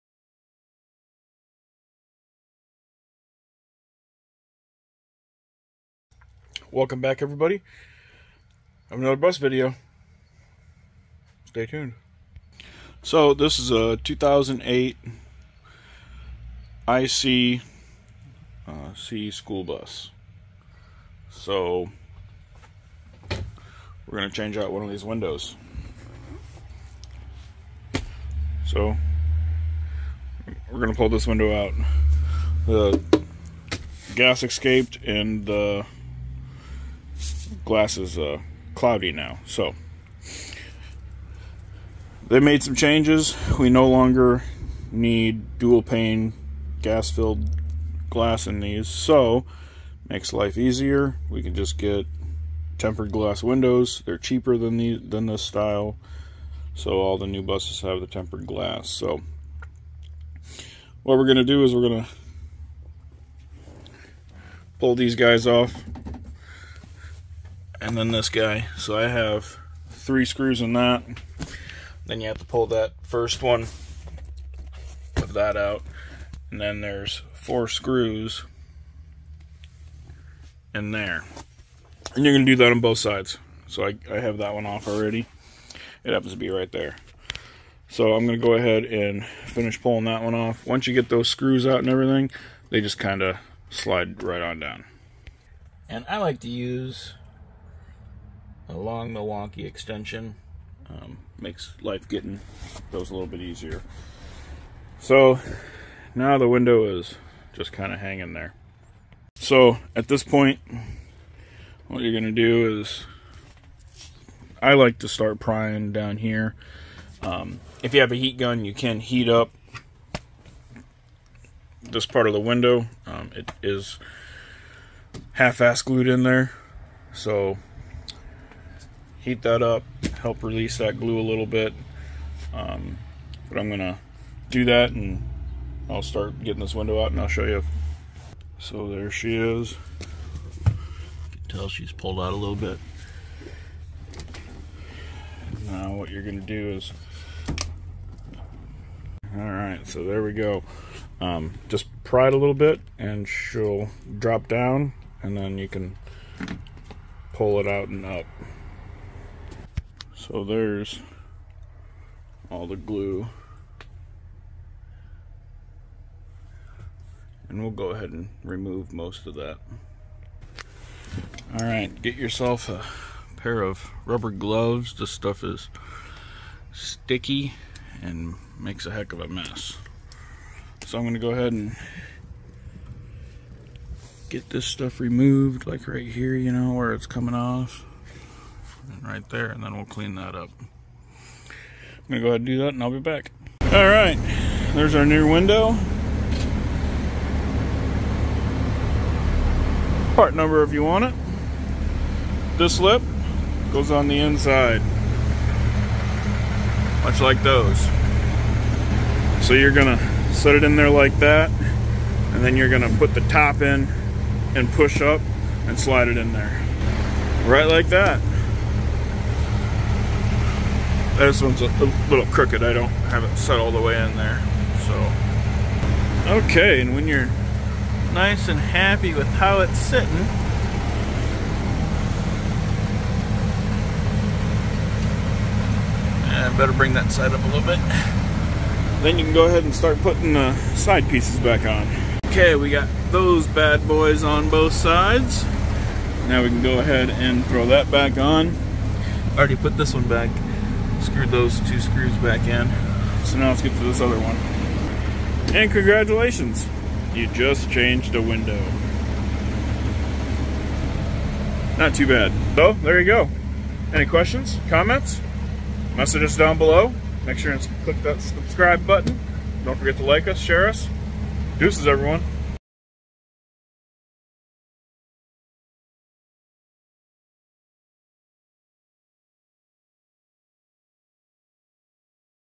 2008 IC CE School Bus Window Replacement
We are replacing the passenger windows in this 2008 IC CE School Bus.